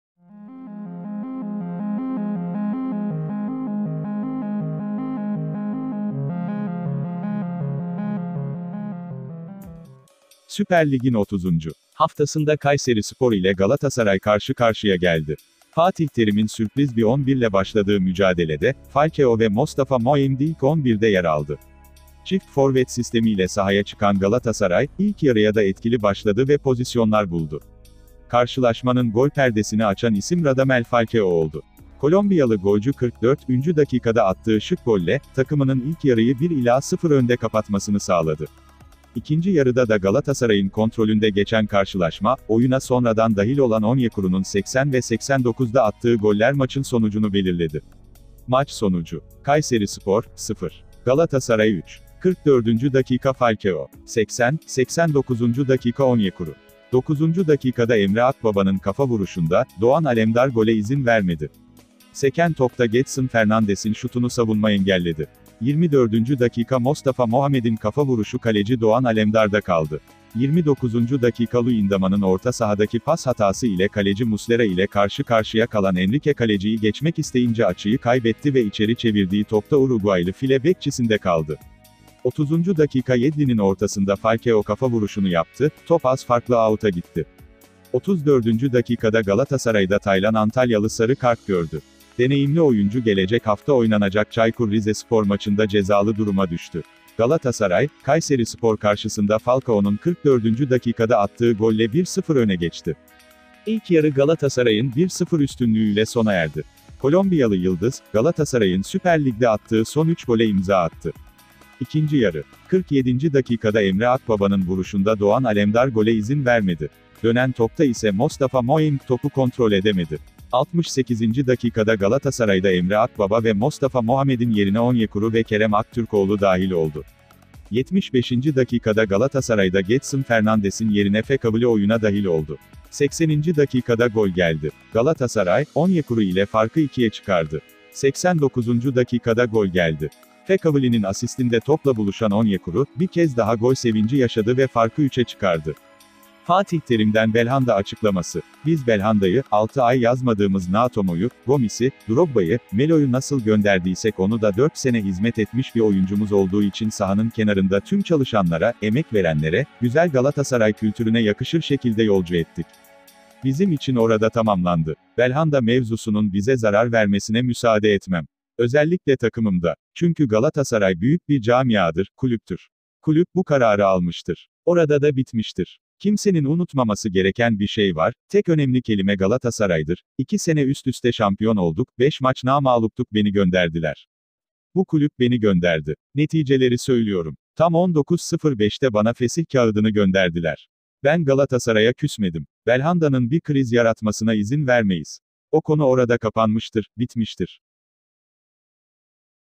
Galatasaray Kayserispor Maç Özeti, Anlatımlı
Galatasaray KayseriSpor Maç Özeti, Anlatımlı, Futbol haberleri, Süper Lig’in 30. Haftasında Kayserispor’a konuk olan Galatasaray, karşılaşmadan 3-0 galip ayrıldı. Sarı-kırmızılılara galibiyeti getiren goller 44’de Falcao, 80 ve 89’da Onyekuru’dan geldi. Ligde oynadığı son 2 maçta puan kaybeden Galatasaray, Kayseri deplasmanında aldığı galibiyetle 61 puana yükseldi. Ev sahibi Kayserispor ise 31 puanda kaldı.